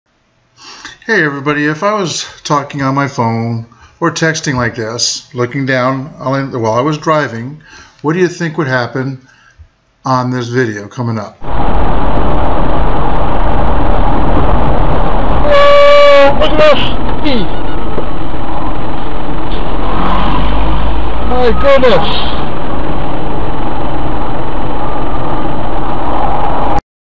Near Head on with a Truck
This is why you need to pay attention while driving.